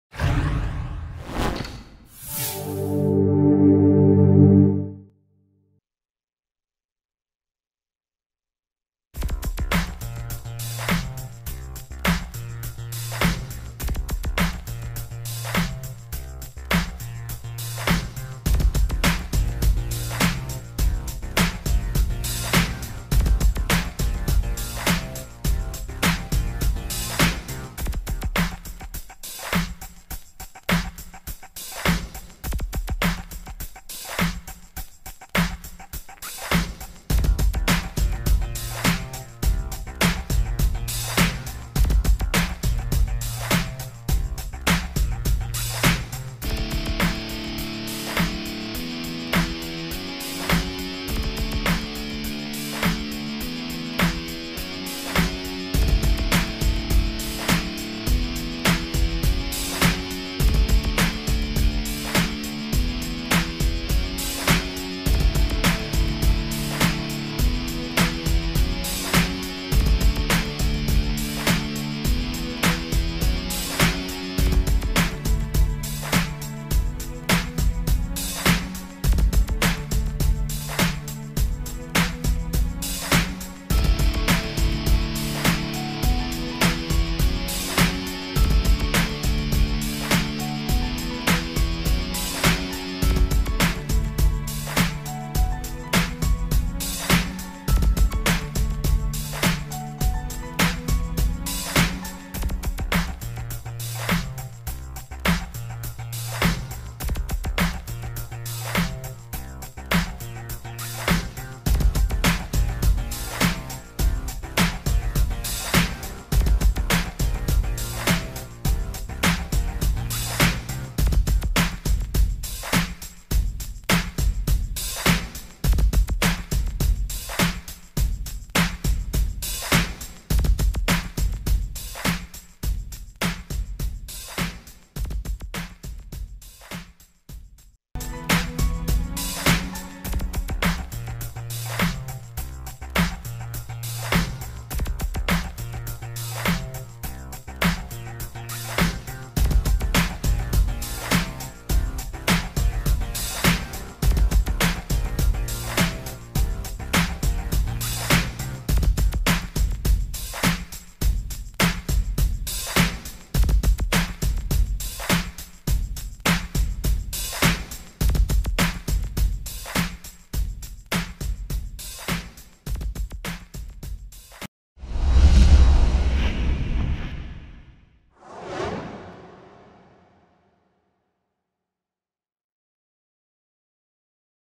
Highlights: Men's Basketball vs Belmont 1/30/20
Final:
Belmont-92
Tennessee Tech-84